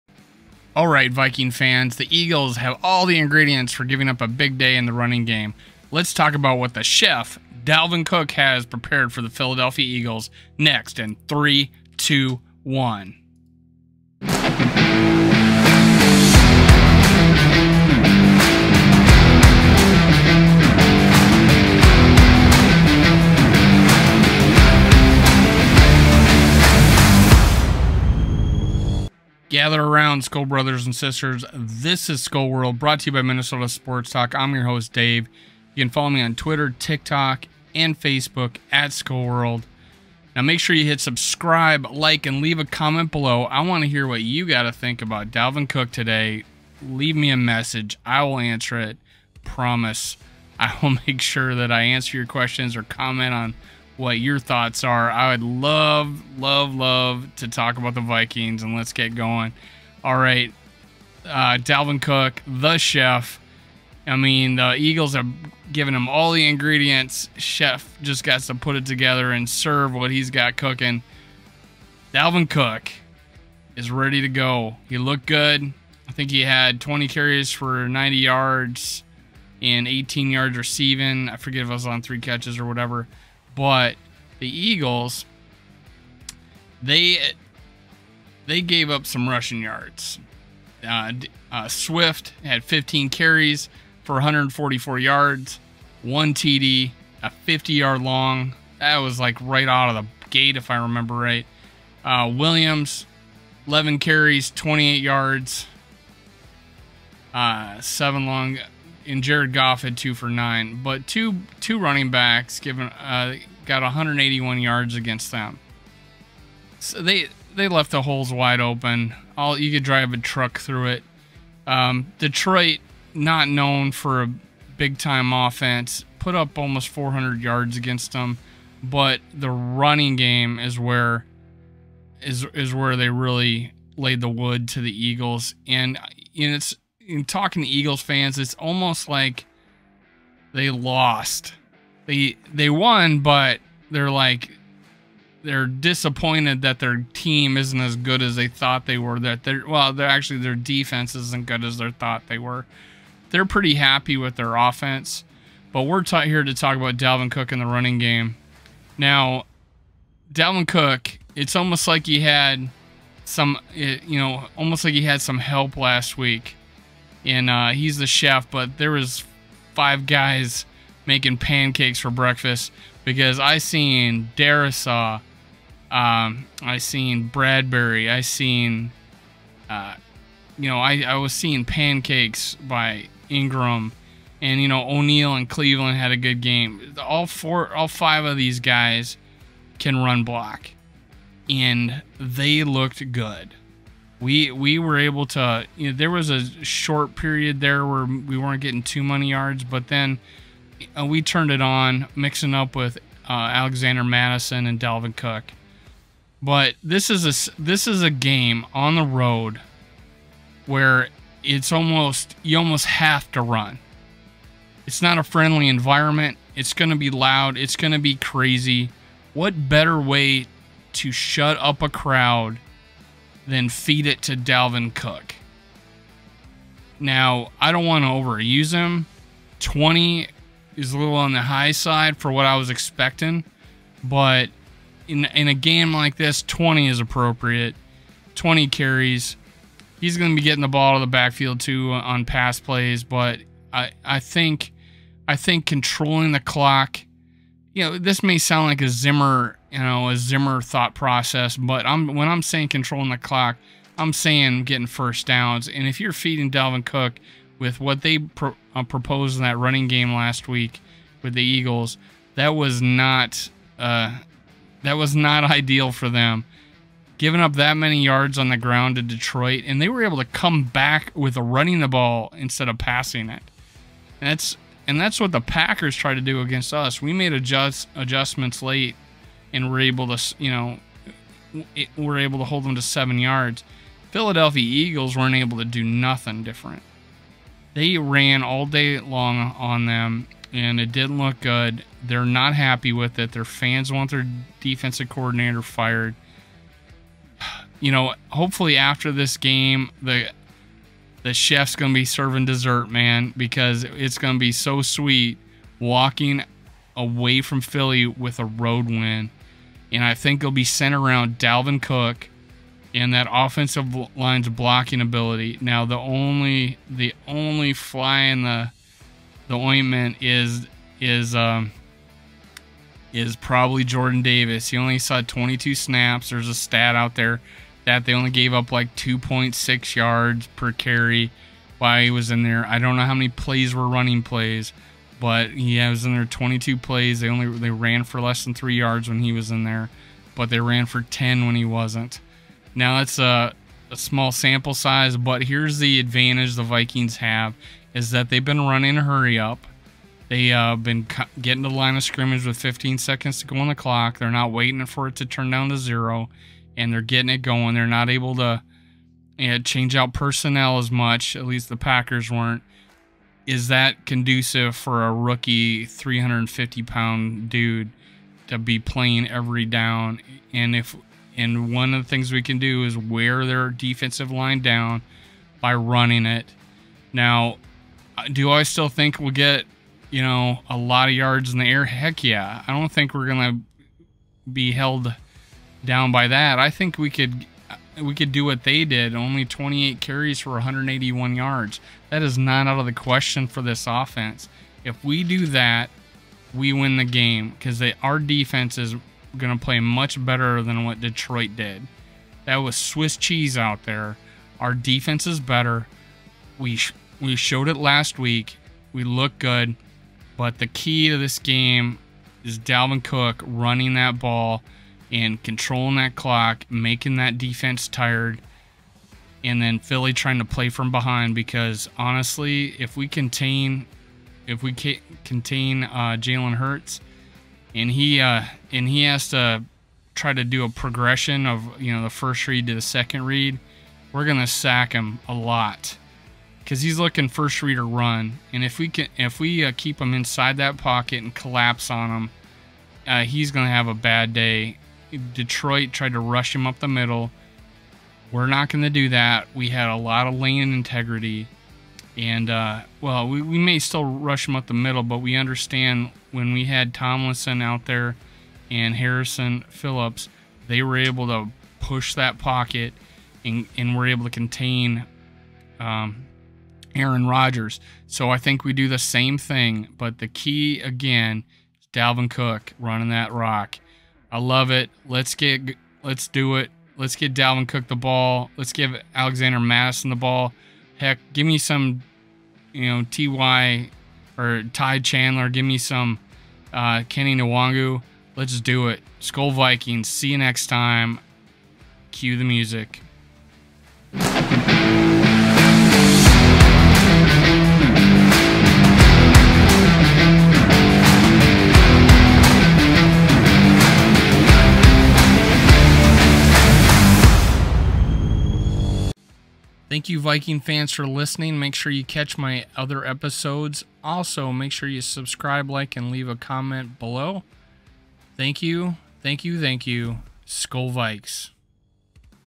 What Is the Chef Going to Serve the Eagles?
I discuss how important Dalvin Cook will be during the matchup with the Eagles.